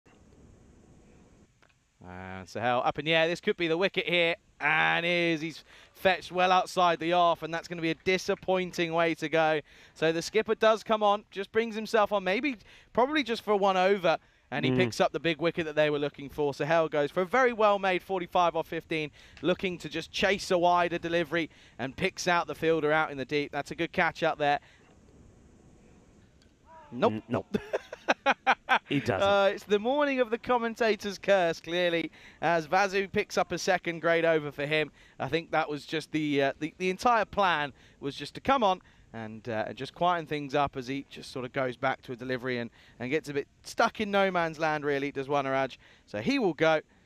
POTM Match 31: V.Saini - ZIN vs CLJ | ECS Romania, 2024 | 7 Jul 2024 | ECS24.600
Player of the Match: Vasu Saini (2/3) for Cluj vs Zinitis | Highlights | ECS Romania, 2024 | 7 Jul 2024 | ECS24.600